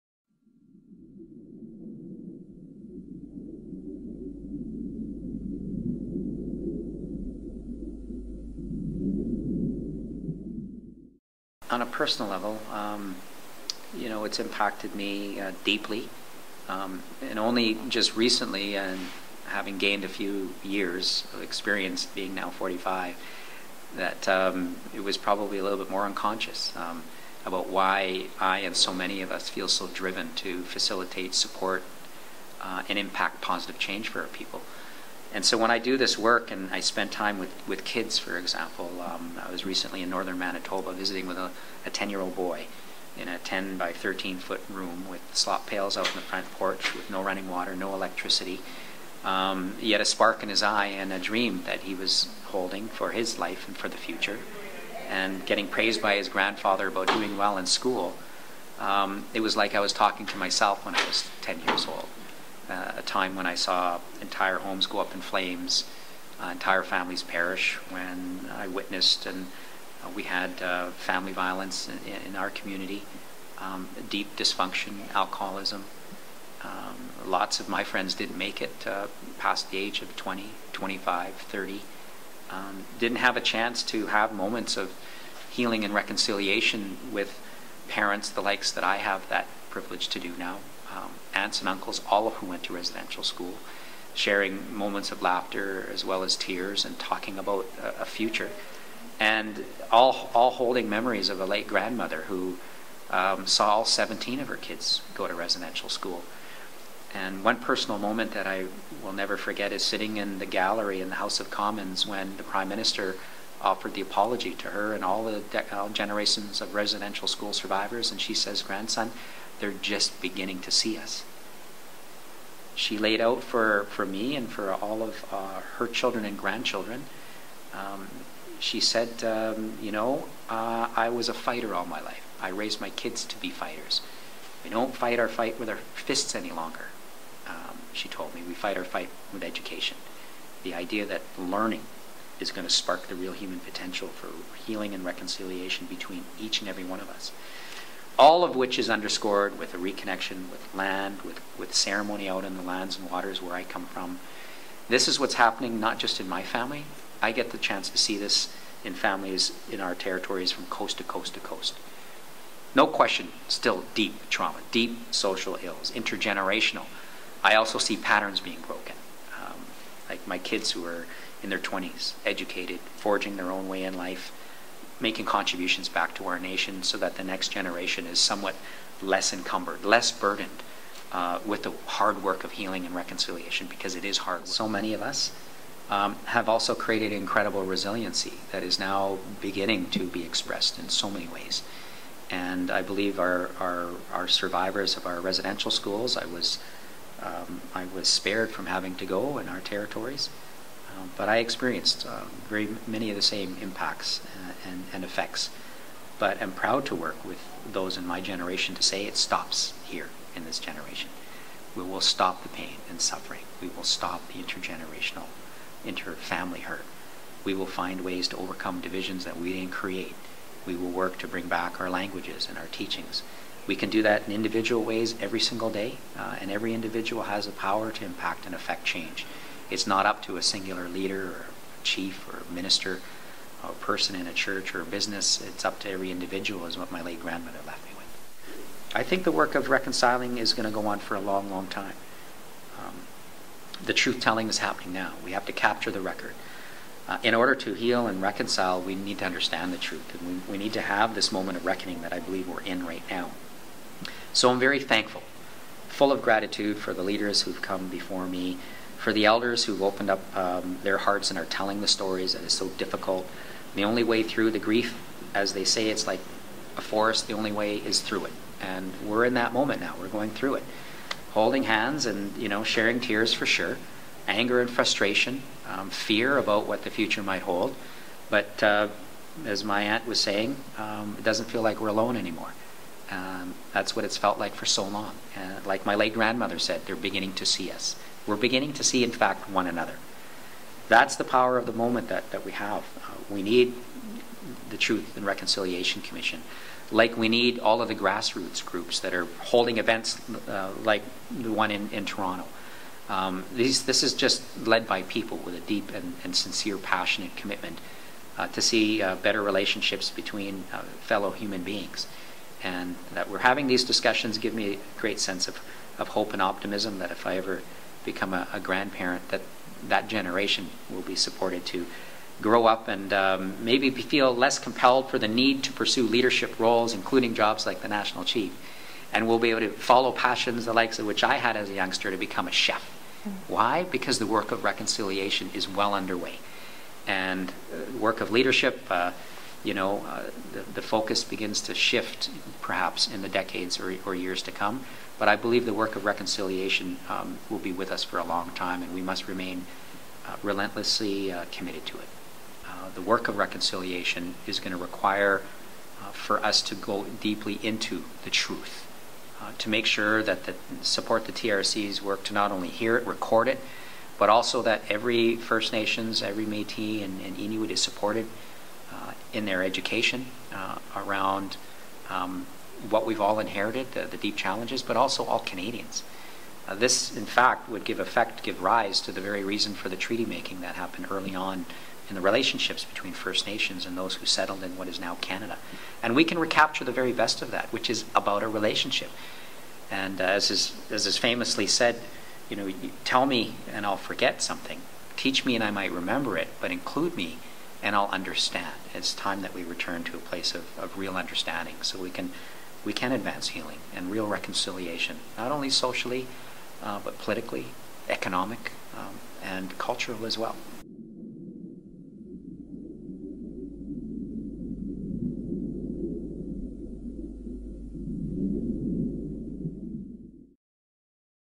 Shawn A-in-Chut Atleo
At THE MEETING PLACE - truth and reconciliation  gathering hosted by Toronto Council Fire Native Cultural Centre in June 2012, Assembly of First Nations National Chief Shawn Atleo shared his insights about the impacts of the truth and reconciliation process on him and his family, as well as in the First Nations communities he's visited from coast to coast to coast.
National Chief Shawn Atleo

Shawn A-in-Chut Atleo is the National Chief of the Assembly of First Nations in Canada. Atleo is a First Nations activist and formerly served as the AFN's Regional Chief in British Columbia. He is a Hereditary Chief of the Ahousaht First Nation, part of the Nuu-chah-nulth Nation and holds a Master of Education in Adult Learning and Global Change (MEd) from the University of Technology in Sydney, Australia. In 2008, he was named Chancellor of Vancouver Island University in Nanaimo, British Columbia, becoming the first university chancellor of Aboriginal heritage in the province's history.

Atleo has been the executive director of a family addictions treatment facility and of an Aboriginal post-secondary training institute, Umeek Human Resource Development. He was a participant in the United Nations Declaration on the Rights of Indigenous Peoples and led a delegation to assist in rebuilding Indigenous communities in Indonesia following the 2004 Indian Ocean earthquake and its associated tsunami. He is also a member of the World Future Council.

Atleo and his partner of 26 years, Nancy, have two adult children, Tyson and Tara.